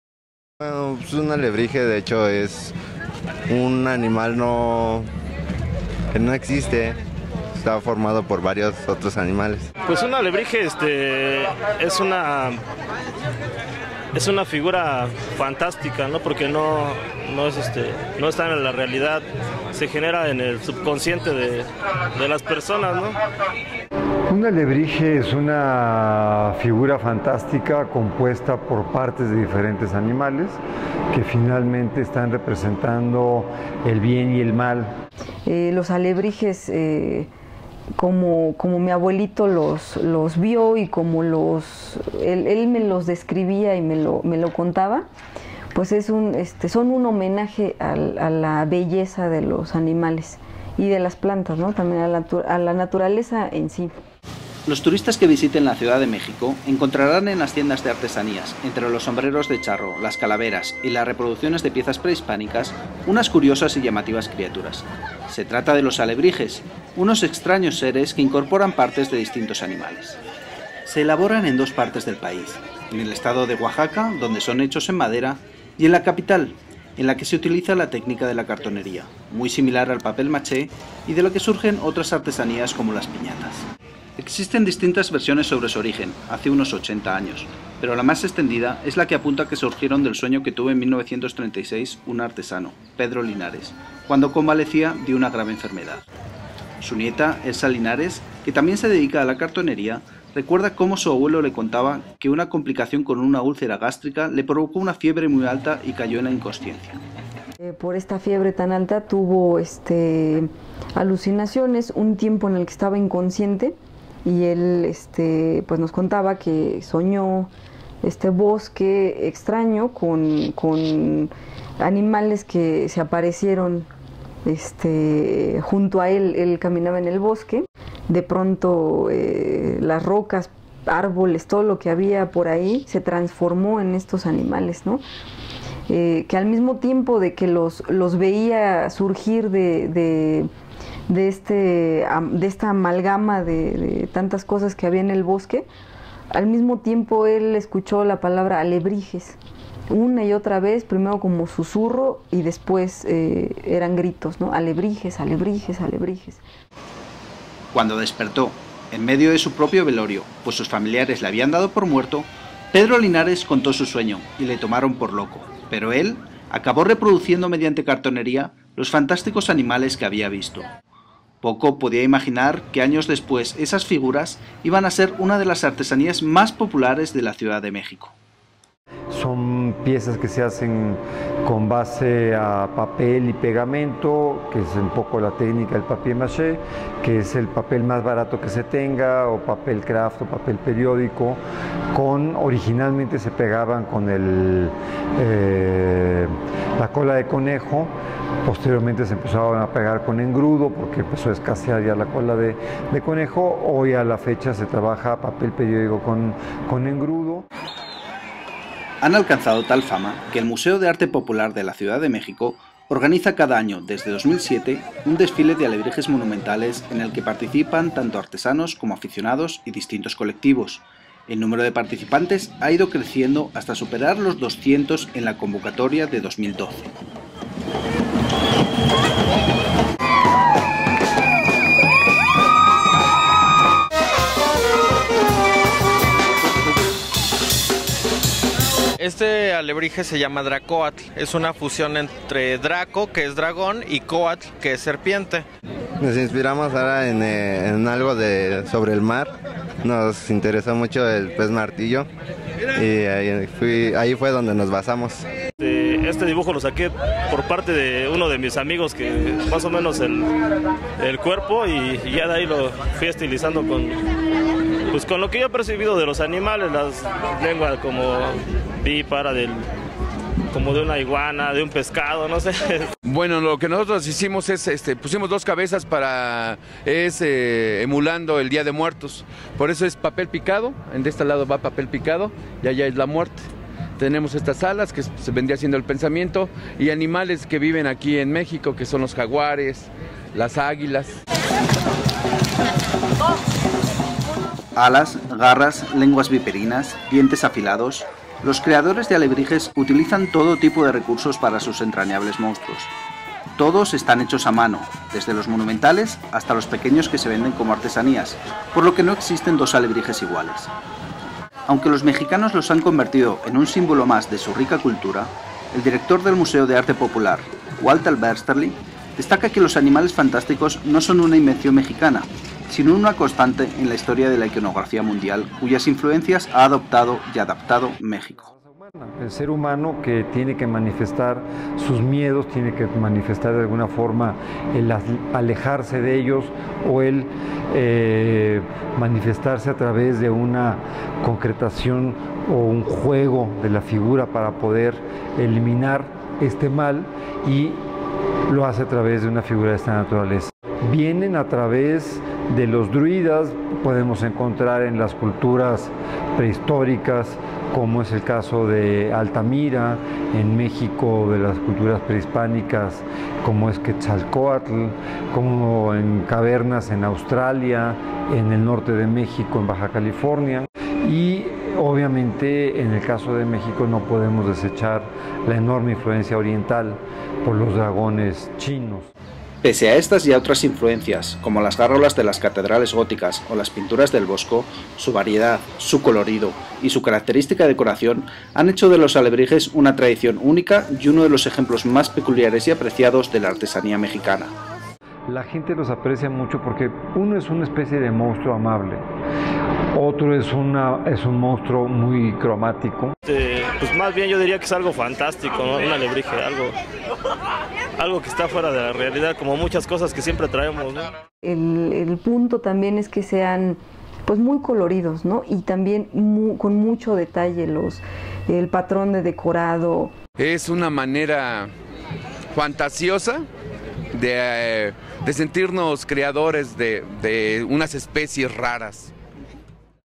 Crónica Errática: Los alebrijes, las fantásticas criaturas de la artesanía mexicana
Los alebrijes son unas curiosas artesanías mexicanas consistentes en unas coloridas criaturas fantásticas formadas por partes de distintos animales y que son típicas de la ciudad de México, donde se hacen con la técnica de la cartonería, y de Oaxaca, donde se elaboran en madera.